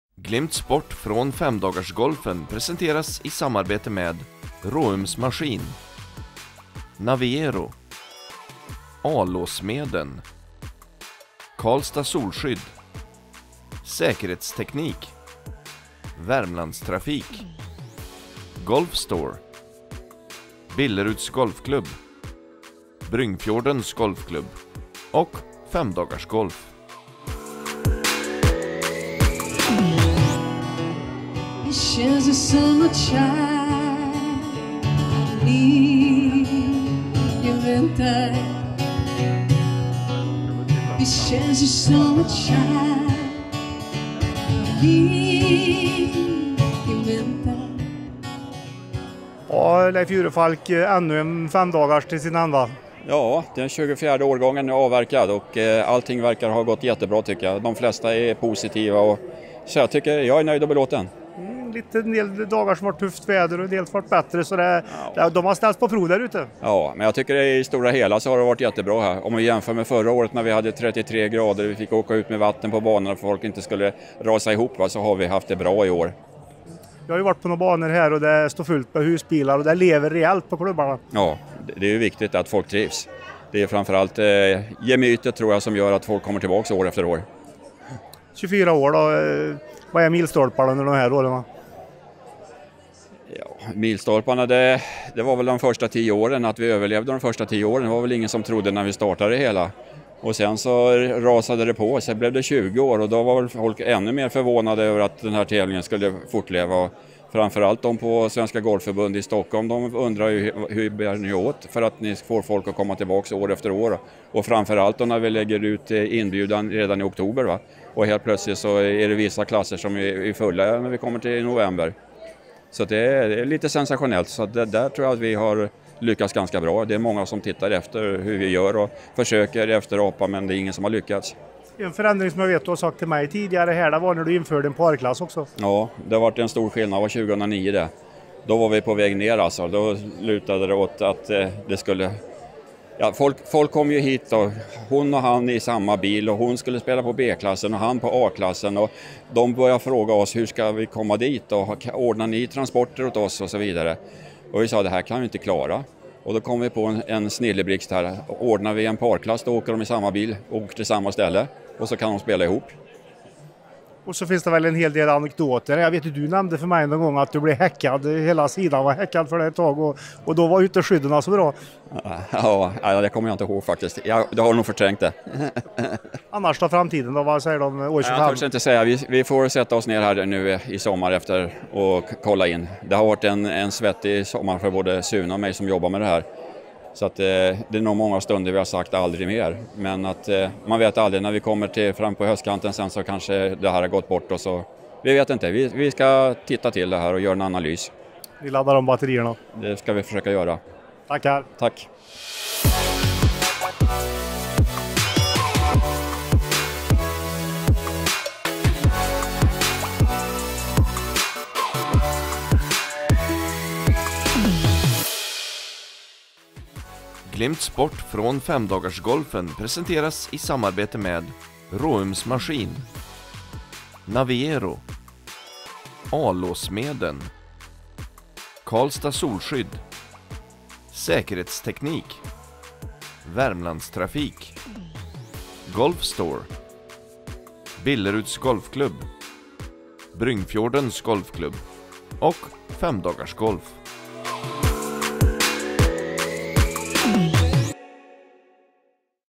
Några ord med Femdagarsgeneralen
På banketten efter årets Femdagarsgolf fick vi några ord med arrangören Leif Jurefalk. Årets Femdagarsgolf var den 24:e i ordningen och avgjordes på golfbanorna Karlstad, Bryngfjorden, Billerud, Kil och Hammarö.